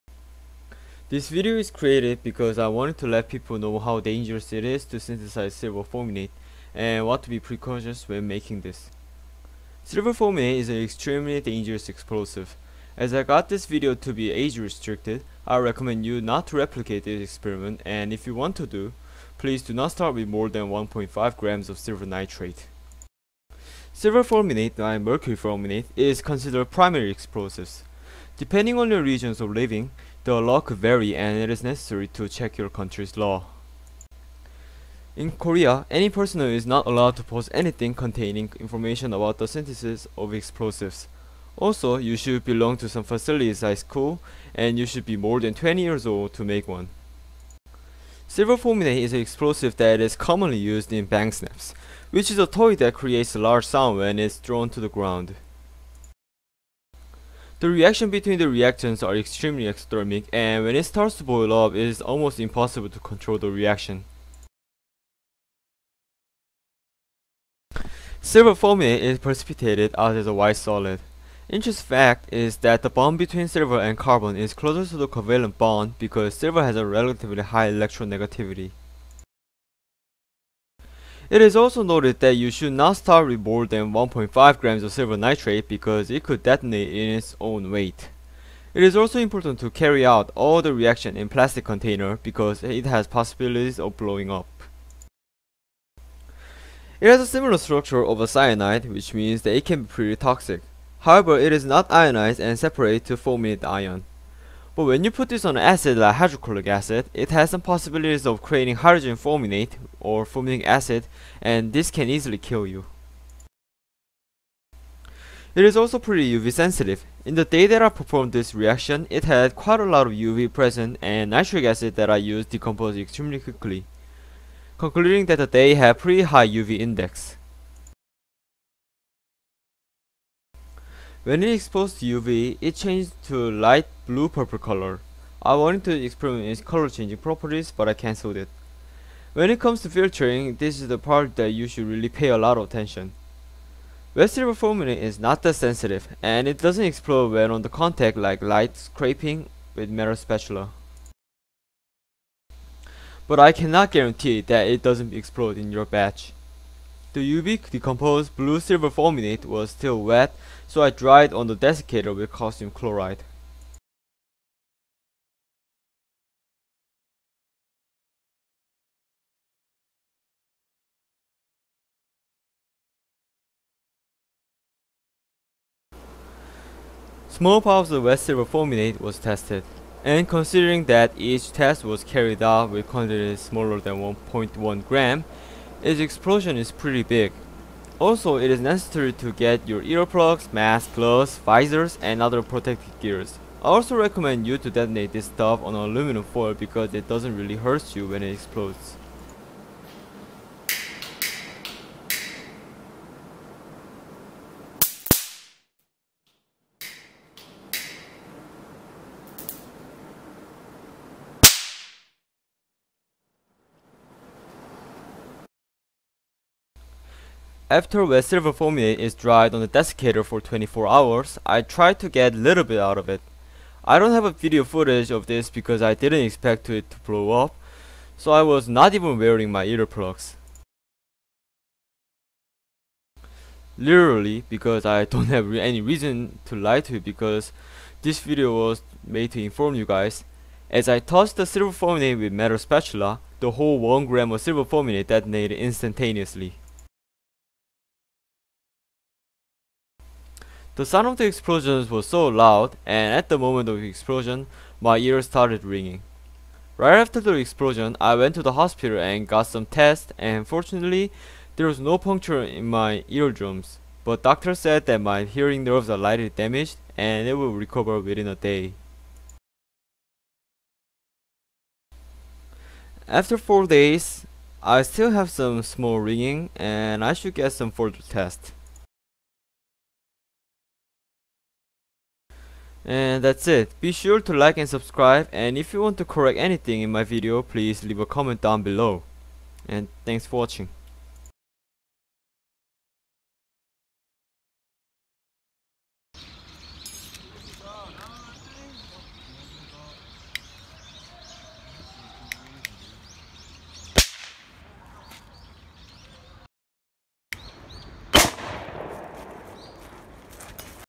Silver Fulminate: Precautions and Warning
What to be precautious when making Silver fulminate or any other fulminate based explosives.

This video is intended to inform viewers.
If you don't know what you're doing,  don't even try to make it.